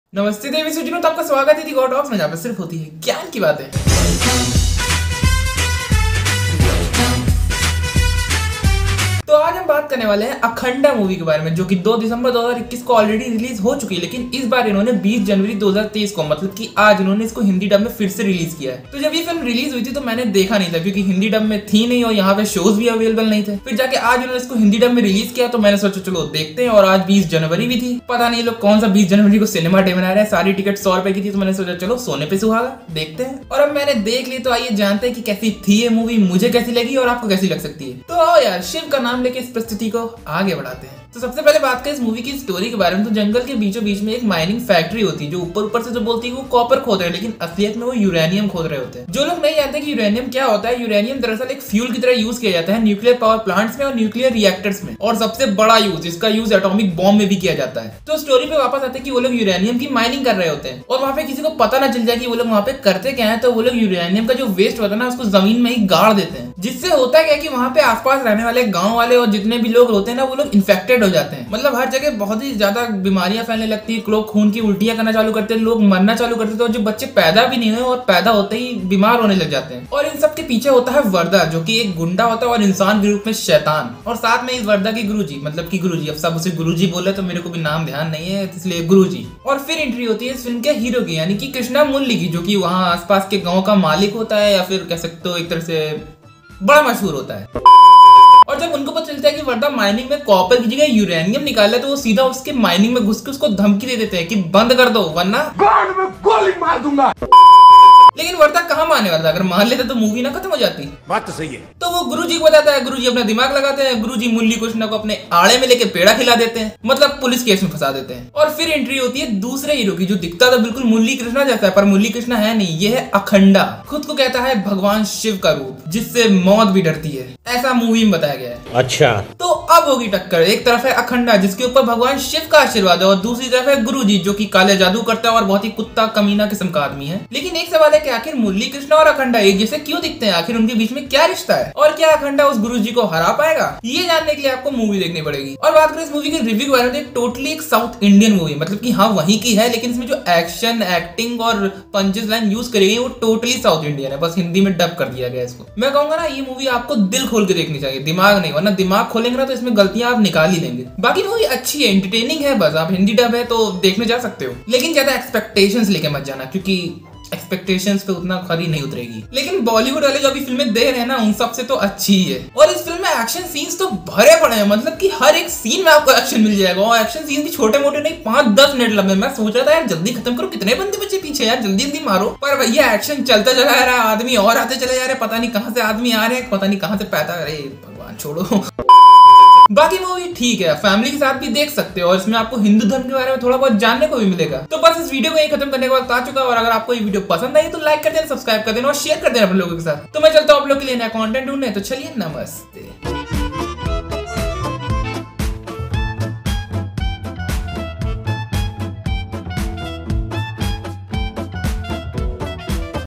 Akhanda Movie Review | THE GAUR TALKS
To aaj ham baat karne wale han Akhanda Movie ke bare ma kyunki aaj eska Hindi Dubbed Version Release hua aur mane usse dekha,

To aoo yaar baat karte han ki akhir kesi thi ye Movie, mujhe kesi lagi aur apko kesi lag sakti ha.

Chapters -
0:00 - Intro
0:41 - Story
2:25 - Review
3:14 - Outro

To ma chalta hun aap logon ke liye naya content dhundne Byeeee.

To jaoo yar video dekho jake yahan kab tak padhte rahoge.